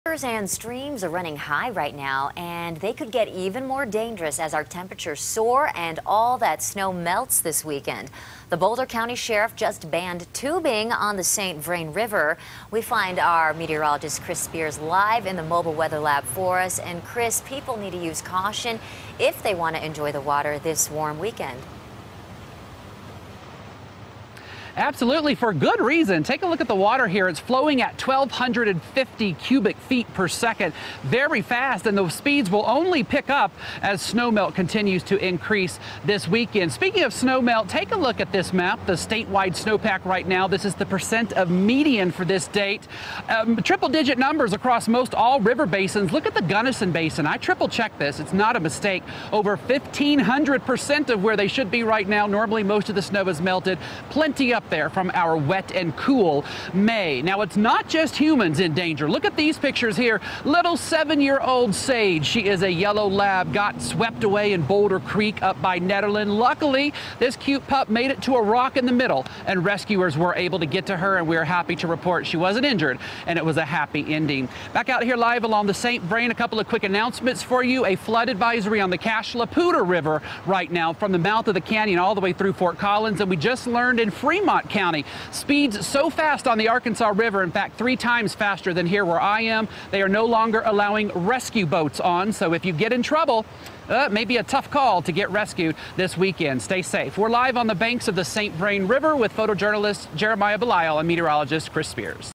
Rushing Streams, Rivers Could Be Dangerous
The Boulder County Sheriff banned tubing on St. Vrain River.